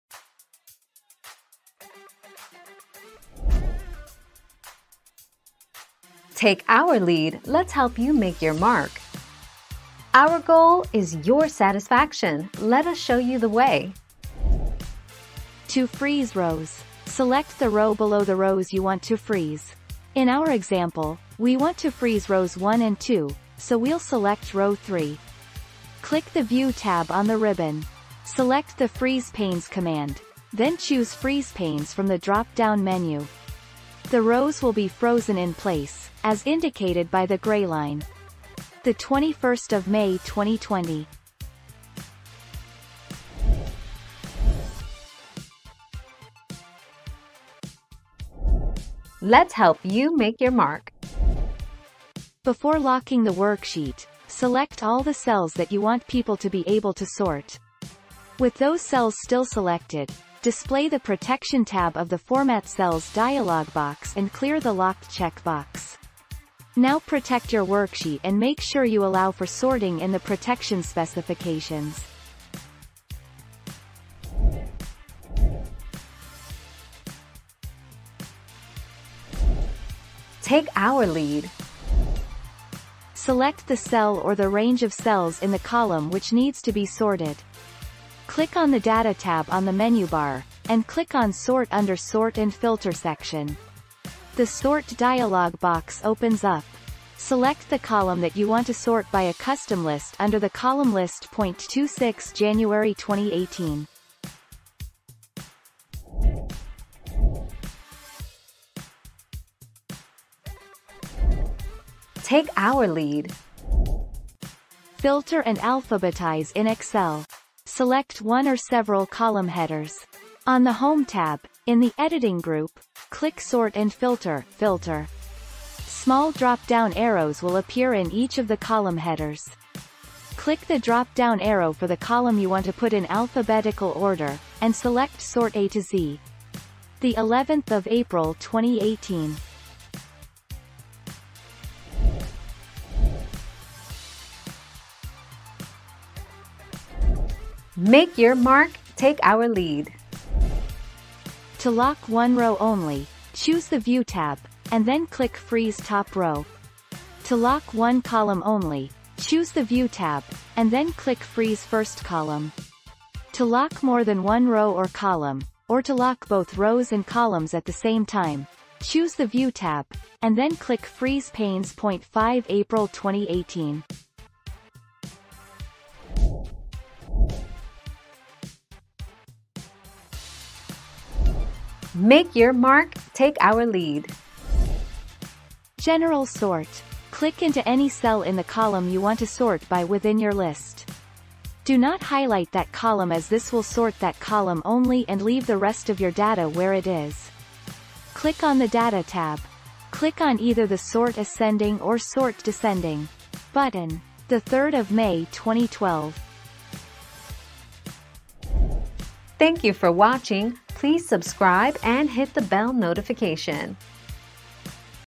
How do you lock rows when filtering?
00:00 - How do you lock rows when filtering?
00:45 - Can you lock cells in Excel and still sort?
01:16 - How do I sort columns without messing up rows?
01:47 - How do you sort A to Z in Excel and keep rows together?
02:23 - How do I lock columns and rows in Excel?
02:57 - How do I sort rows in Excel without mixing data?

Laura S. Harris (2021, April 8.) How do you lock rows when filtering?
    AskAbout.video/articles/How-do-you-lock-rows-when-filtering-234499